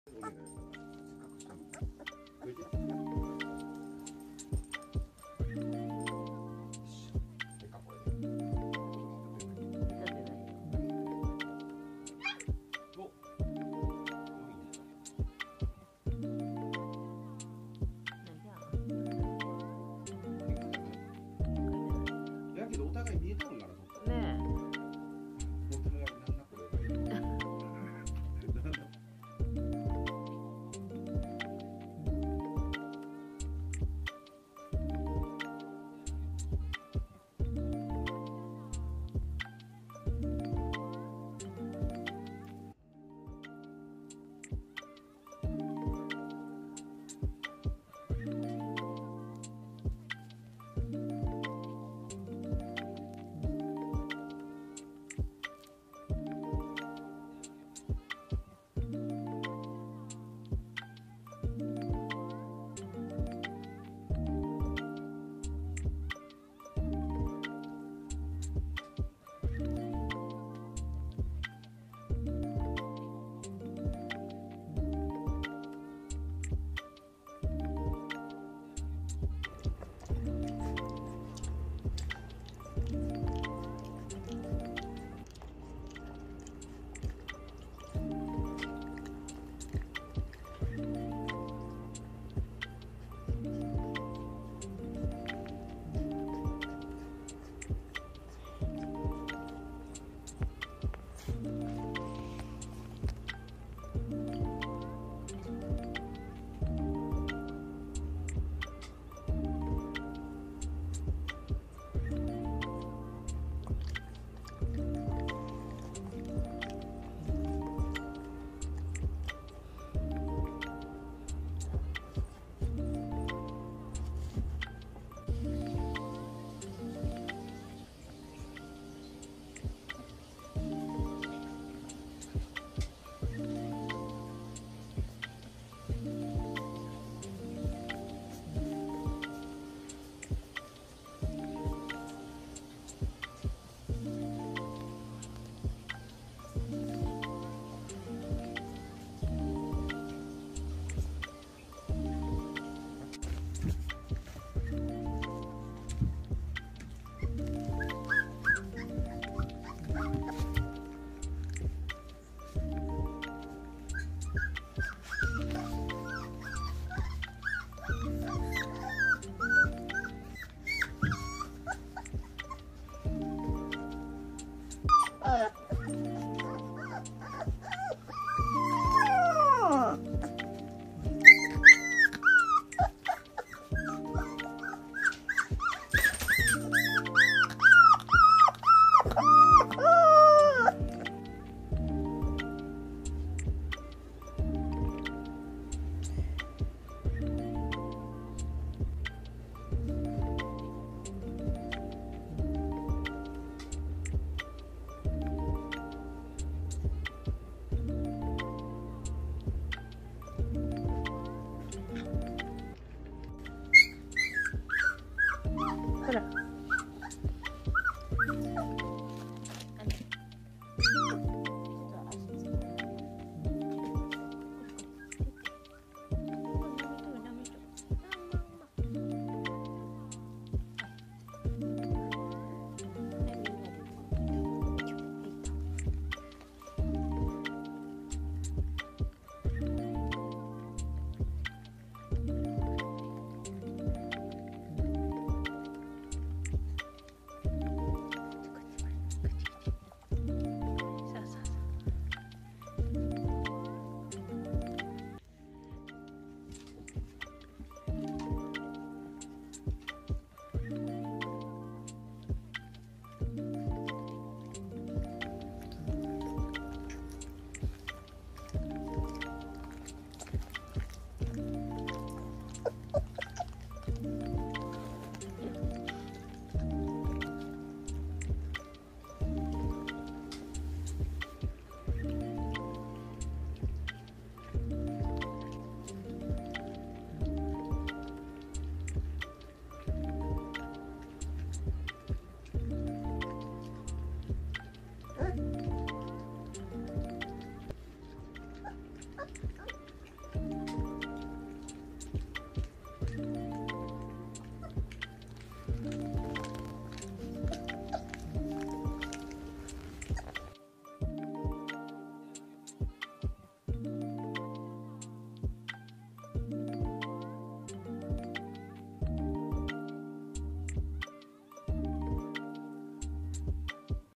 １月３日に生まれたレイラの子どもたち・黒豆ちゃんず♪よちよち歩き、初めての粉ミルク。black labrador baby/puppies
2023年1月3日に生まれたレイラの子どもたちのVlogです😊
この地域では珍しい大雪（大袈裟？）に浮かれて、動画アップするのをすっかり忘れていました・・・